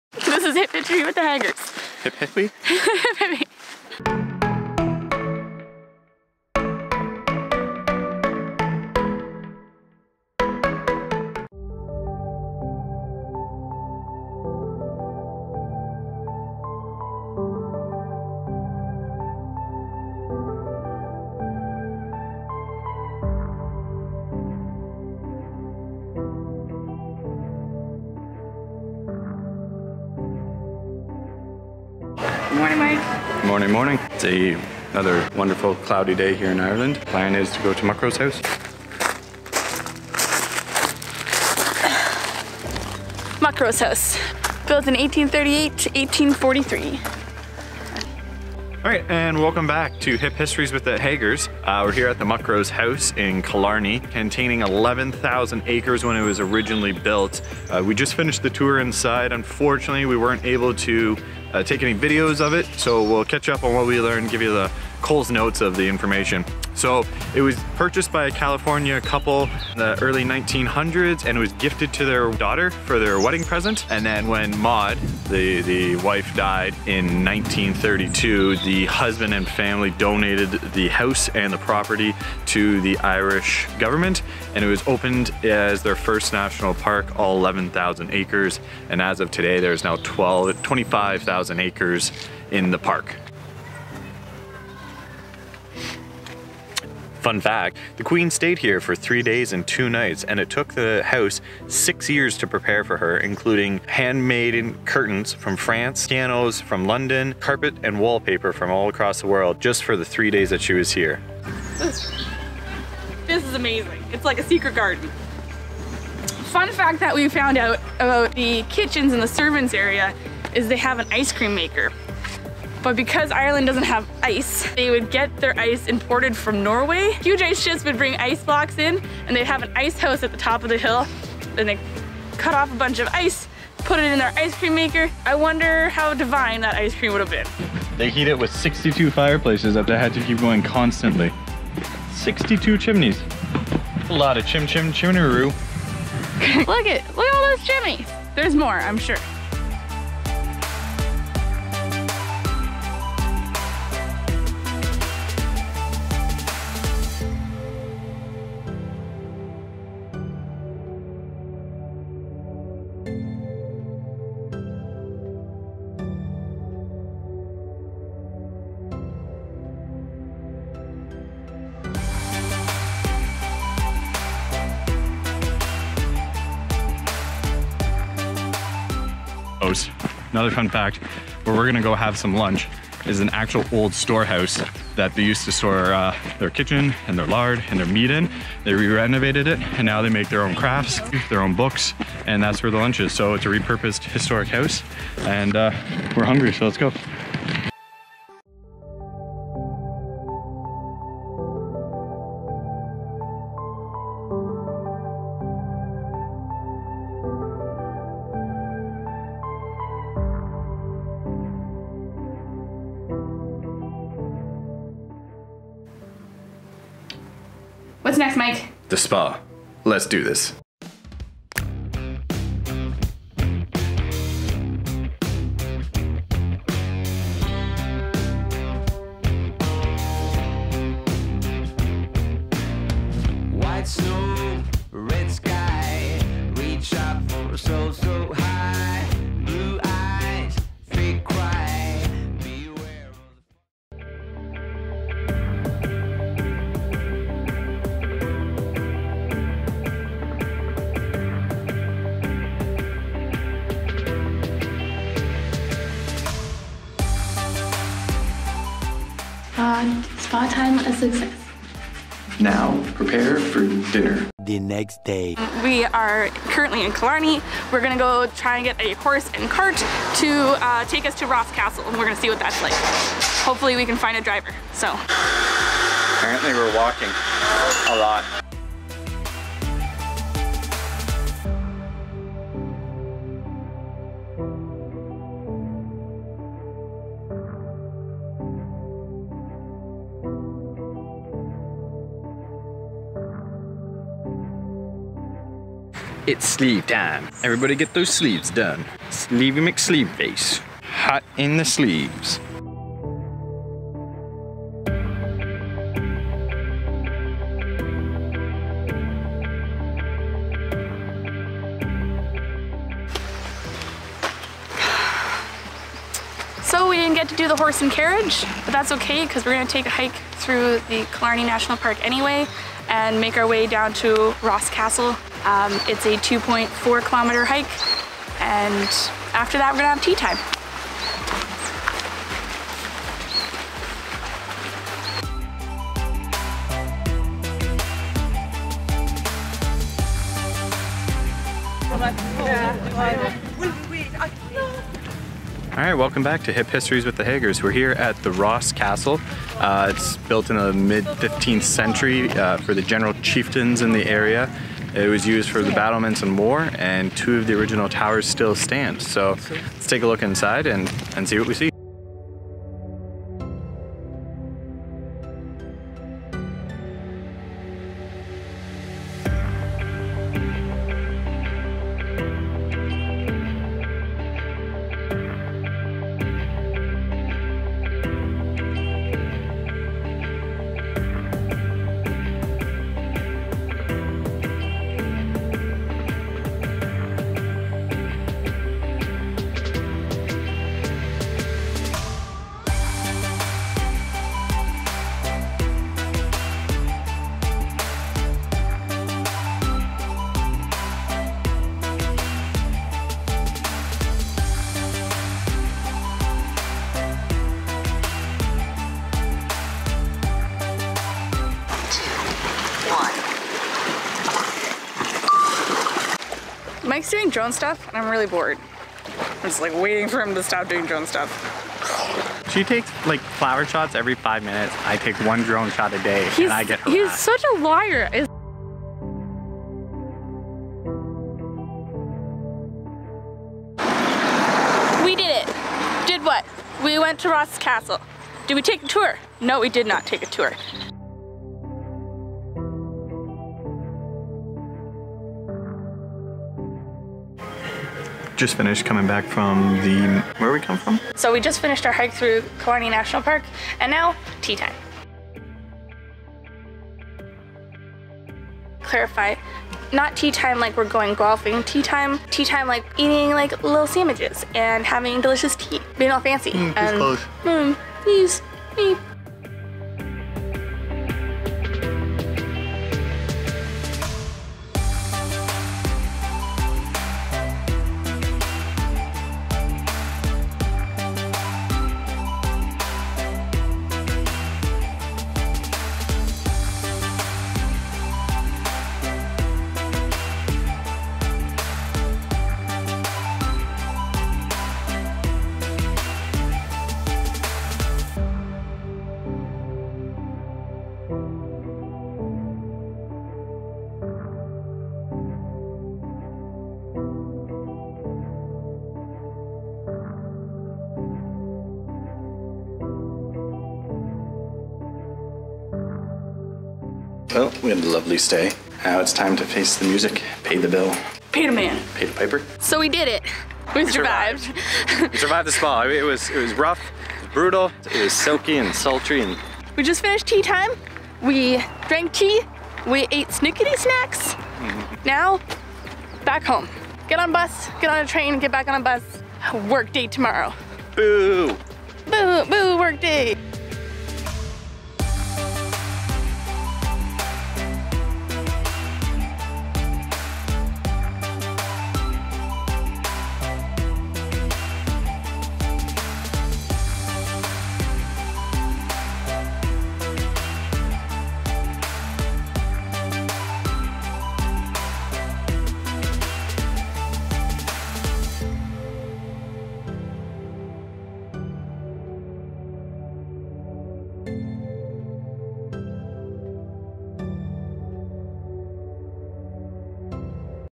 Muckross House + Ross Castle
Our trip in Killarney continues with trip into Killarney National Park to visit the Muckross House and Ross Castle. We enjoy the beautiful day in the park and head out on a hike and finally wrap up our visit with some tea!